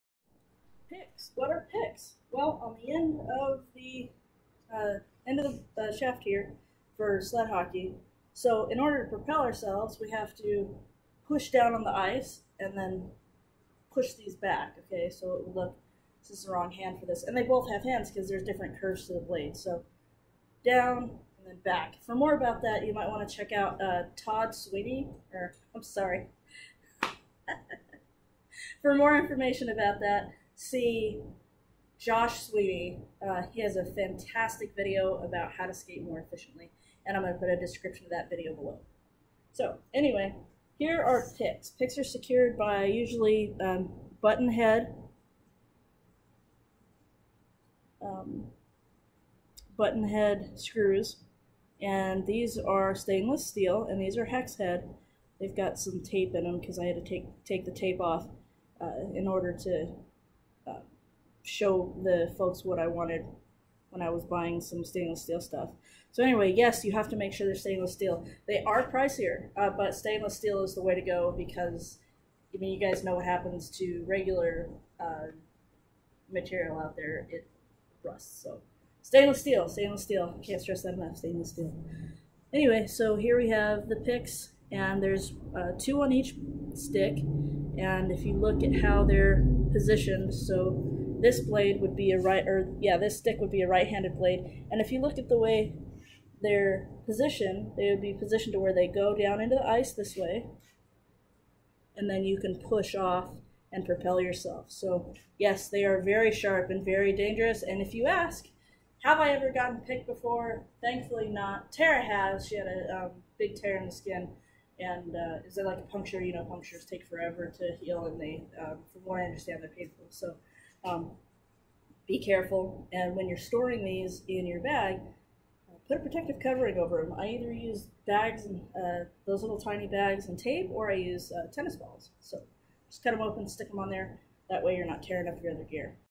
Sled Hockey Equipment - Sticks and Picks
This is a good look up close at what the butt end of our sticks look like to answer the question of how we propel ourselves across the ice.

For information on skating more efficiently search for  "Josh Sweeney skating efficiently." He has some really great videos. Kevin Rempel has also put out some great videos recently.